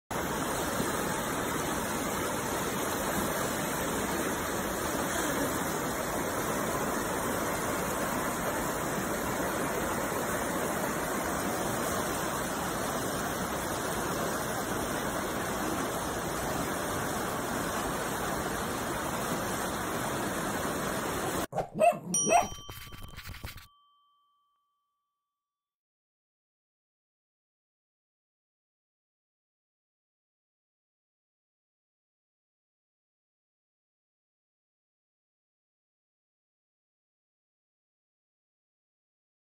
Ducks have fun floating effortlessly down fast-paced river
Check out these adorable ducks riding the waves on Berg Lake trail! They look so awesome!